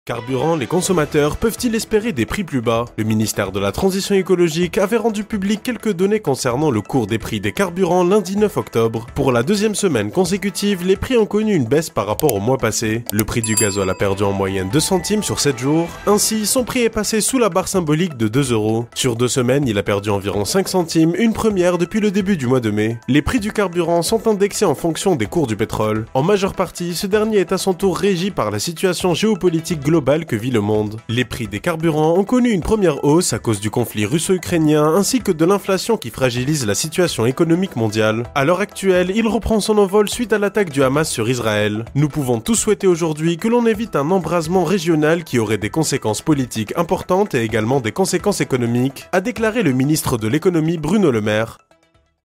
Carburants : Les consommateurs peuvent-ils espérer des prix plus bas ?
Ce début de mois s'annonce favorable pour les Français qui peinent à faire le plein de carburants, tant cette opération est devenue difficile et coûteuse.